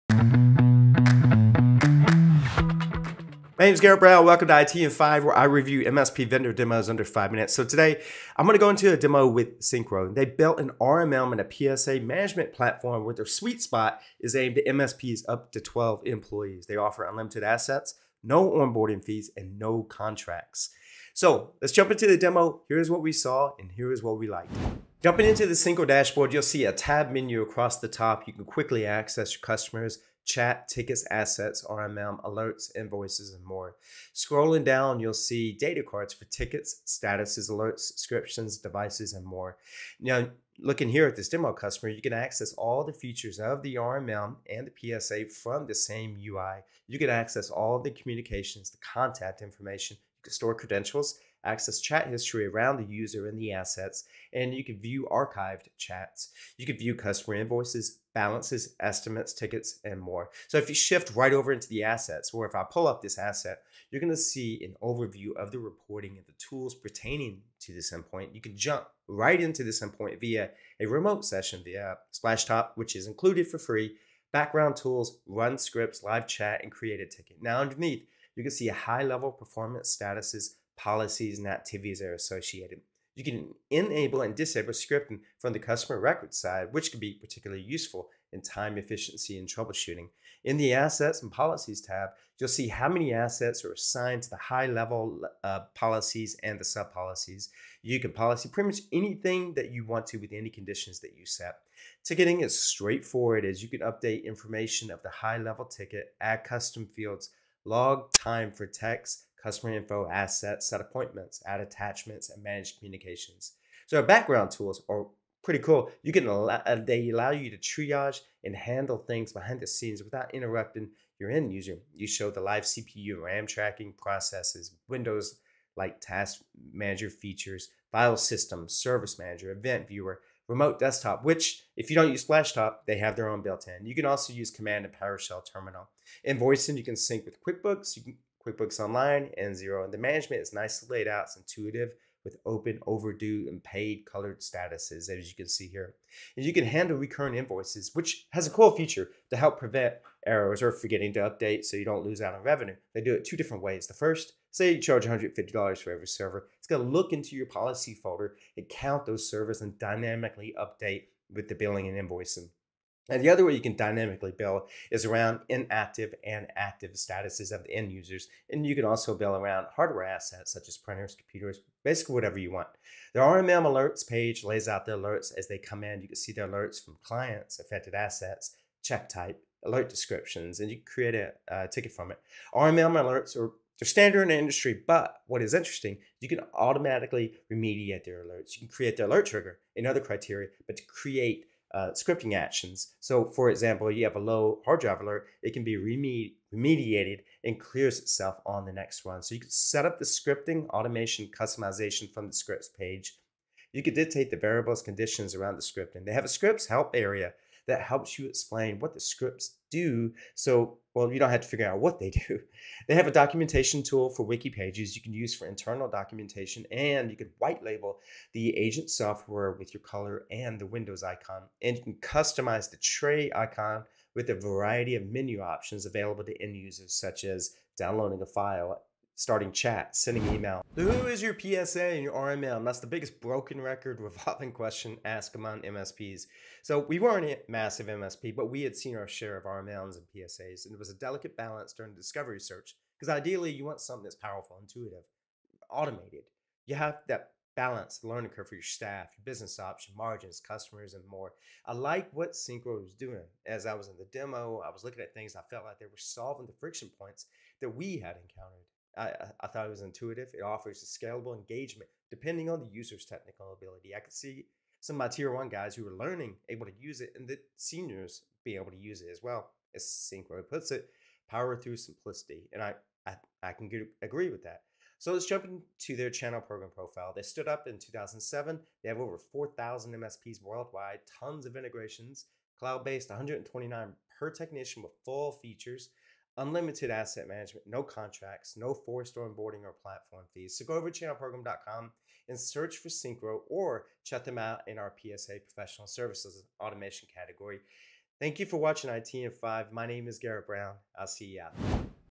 Syncro Review: RMM, PSA & Remote Management Solution
In this installment of our IT in 5 and Under series, Chief Technology Officer and tech uber-enthusiast Garrett Browne reviews Syncro and gives you his take on their RMM, PSA & remote access solution.